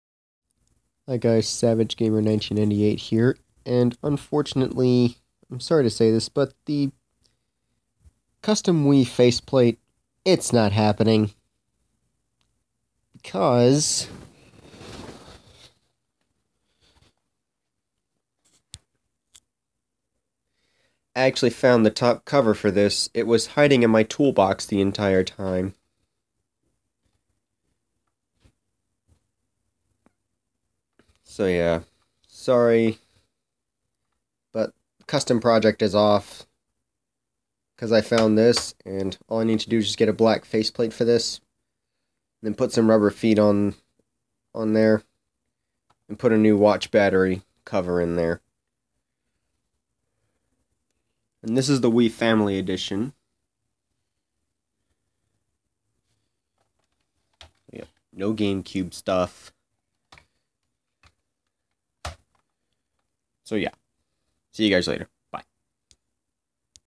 Wii custom project is cancelled. (READ DESCRIPTION)
So basically I found the other half of the black Wii Family Edition, and decided that the project is not happening. So I have deleted faceplate scheme video.